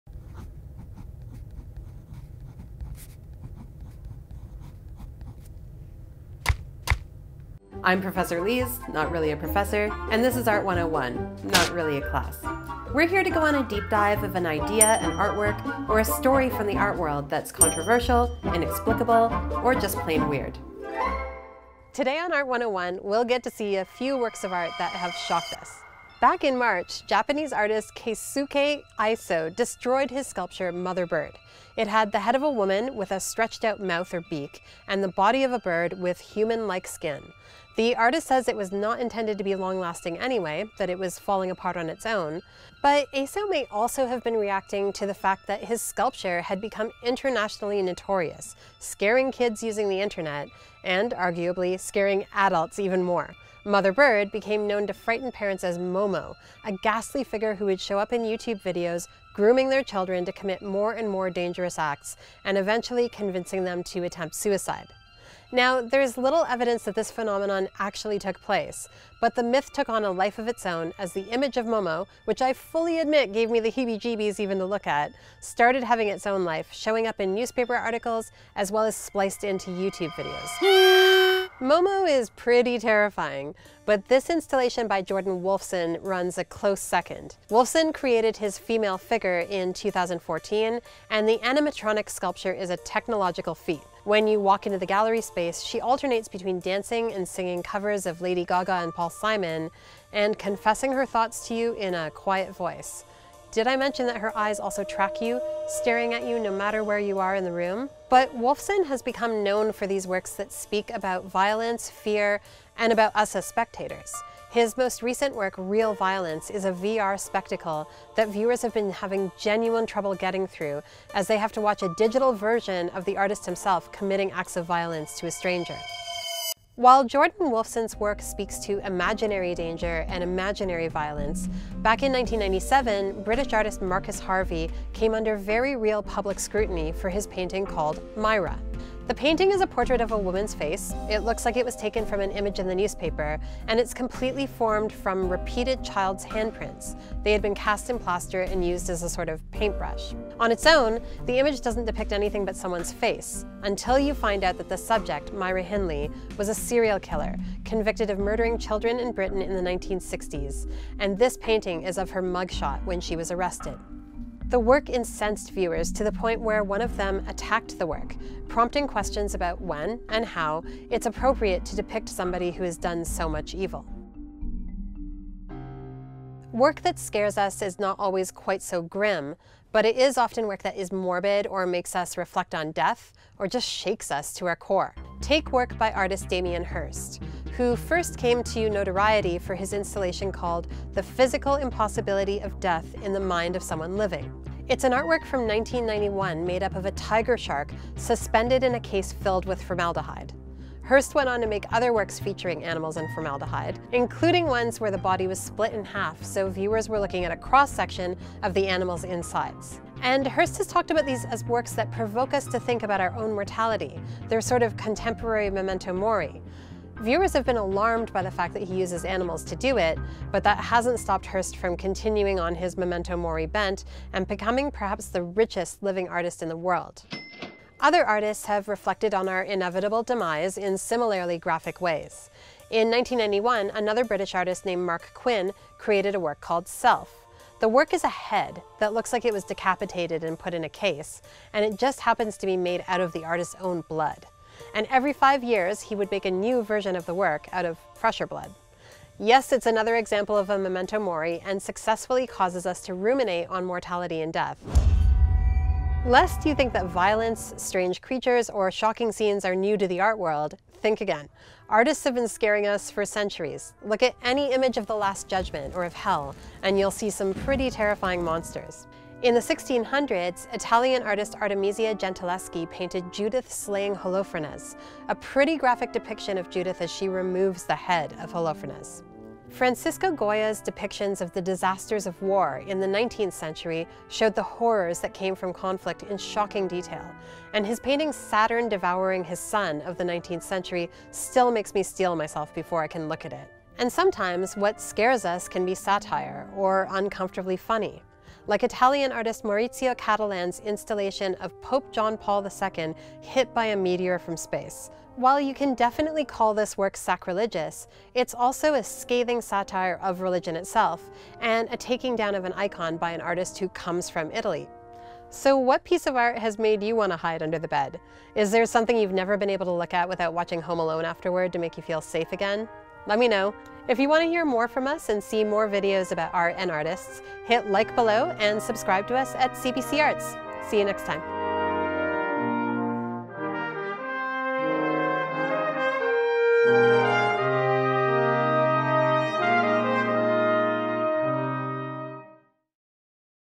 Terrifying art that makes you want to hide under the bed | Art 101
Prepare yourself to learn about the long history of terrifying art from animals preserved in formaldehyde to a head made from the artist's own blood.

**FULL DESC**